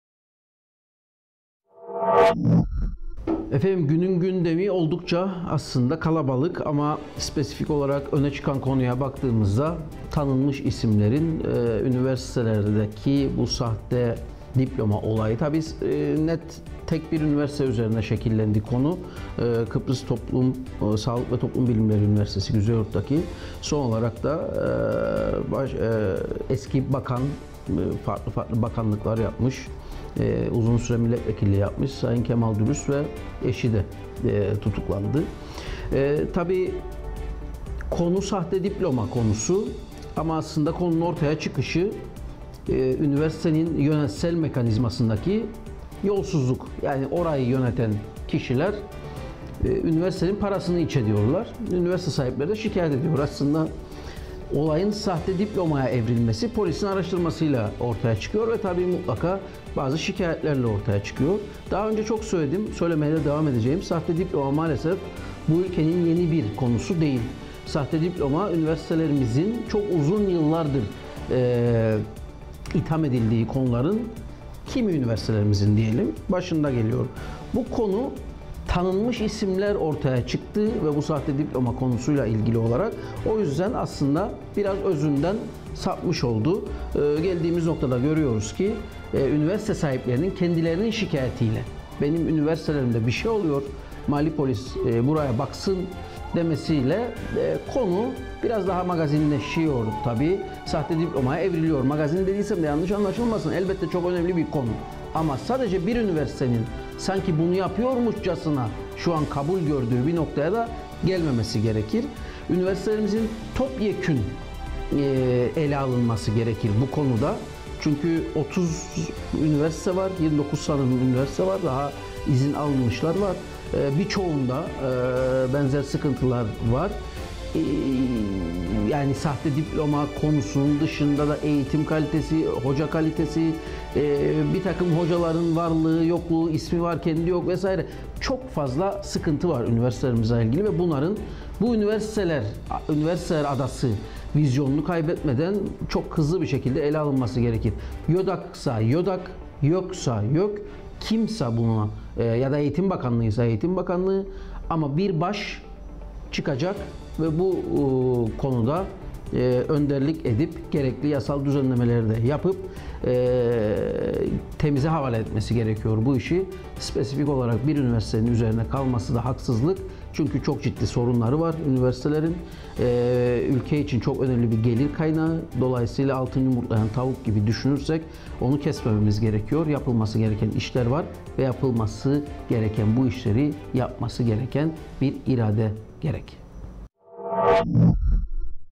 Aytuğ Türkkan ile Günün Gündemi
Aytuğ Türkkan ile Günün Gündeminde bugün Kemal Dürüst de tutuklandı.. Üniversitelerin eğitim kaliteleri bir an önce ele alınmalı.  Altın yumurtlayan tavuk elimizden kayıp gidiyor.. bu sadece 1 üniversite konusu değildir.. ilgililer göreve!..